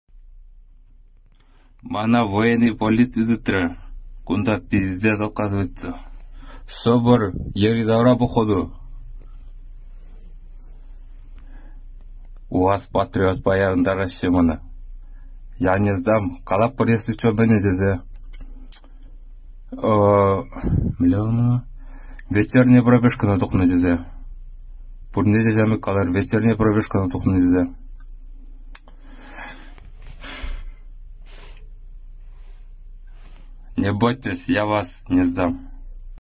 🟠В Ульяновске СОБР задерживает недовольных призывников
Мобилизованные из Чувашии отказываются ехать воевать, пока им не выплатят денежную компенсацию в размере 195 тысяч рублей. Они не хотят идти на войну, зная, что их семьи могут остаться без поддержки.

«Меня военная полиция скрутила. Здесь п...ц оказывается! Кругом бойцы СОБРа. В УАЗ-патриот меня посадили. Я не сдам, если чего скажу, что вышел на вечернюю пробежку. Все говорите, что вышли на вечернюю пробежку. Не бойтесь. Я вас не сдам».